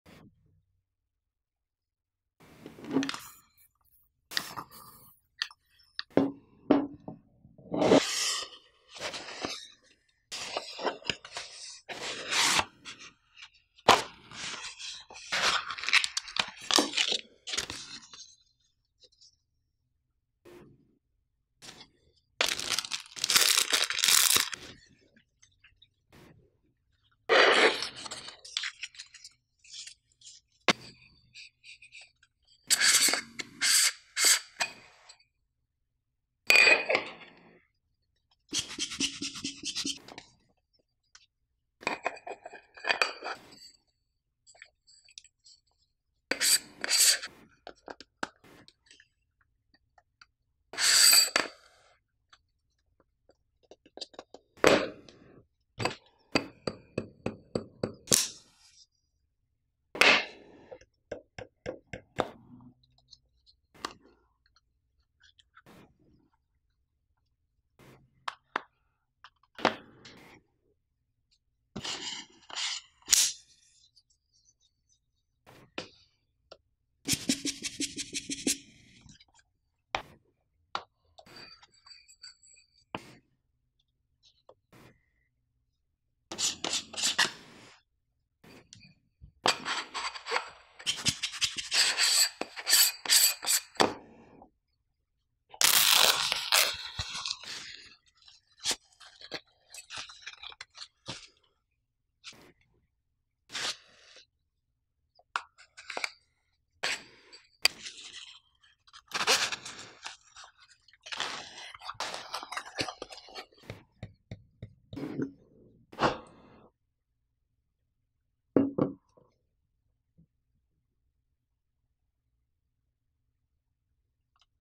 Polar Pro & Peter McKinnon Variable ND -- Unboxing
Unboxing the 2-5 Stop Variable ND Filter from Polar Pro and Peter McKinnon with a cinnamon candle and ASMR

You ready?  Yeah... you're ready